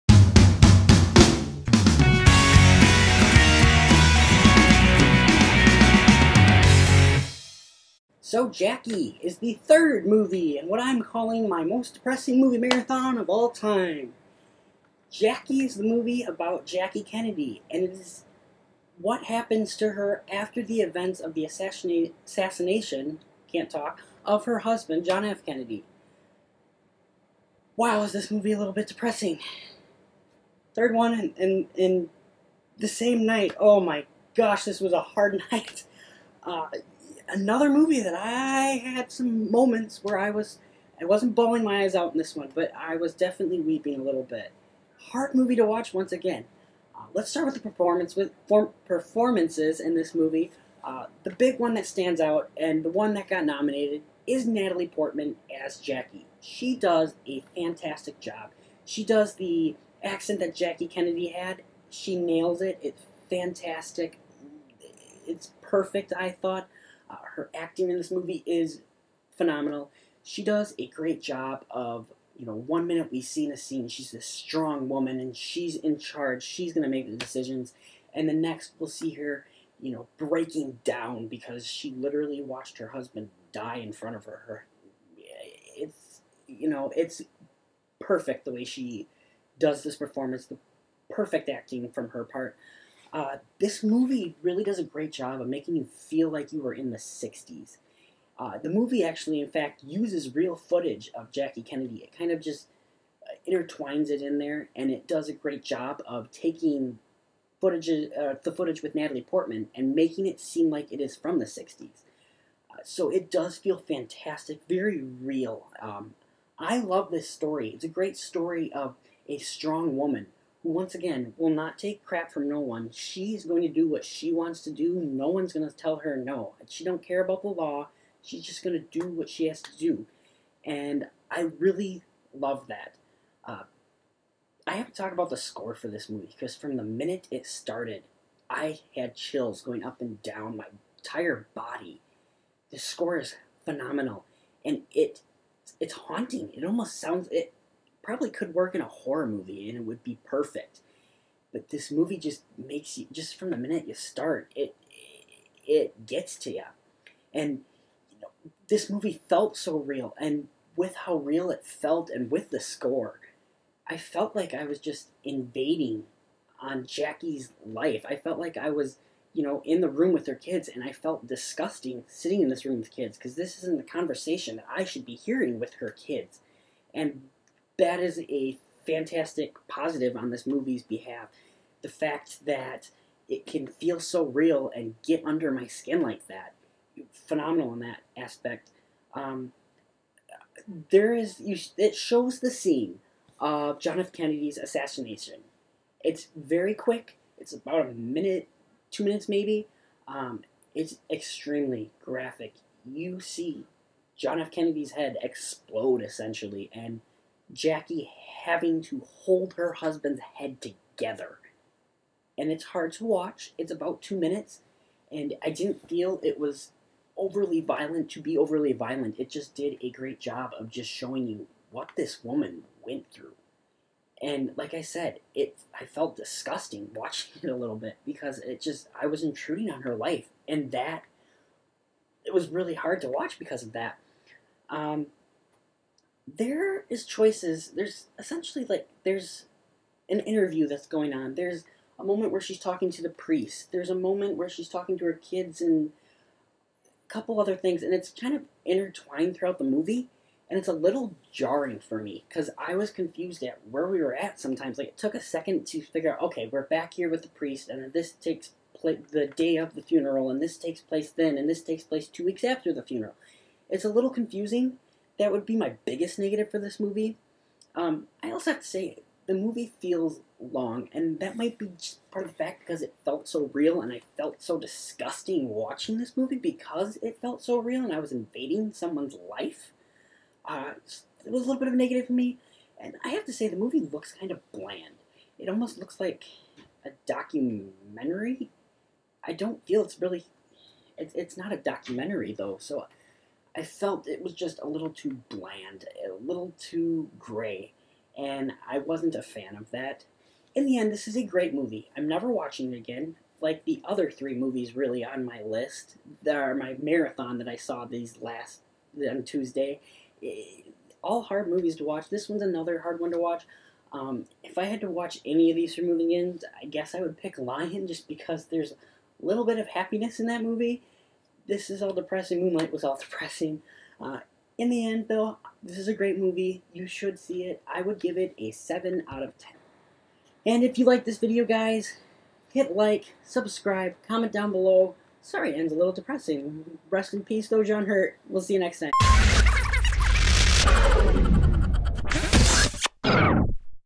Jackie Review
My review of Jackie! My 3rd video in my 3 part depressing movie marathon!

Friend Cody on Snapchat: cjwiese